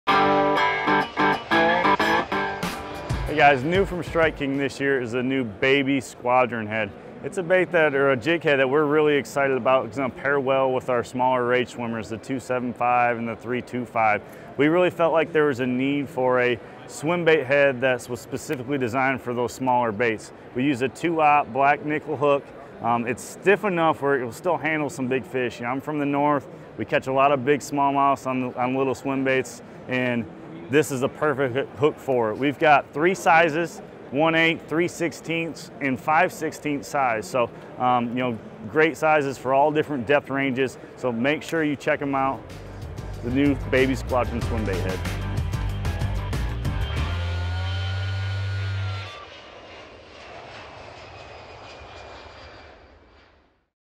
New Strike King Squadron Swimbait Heads with Jonathon VanDam | iCast 2019
TW discusses the New Strike King Squadron Swimbait Heads with Jonathon VanDam at the 2019 ICAST Show in Orlando, Florida.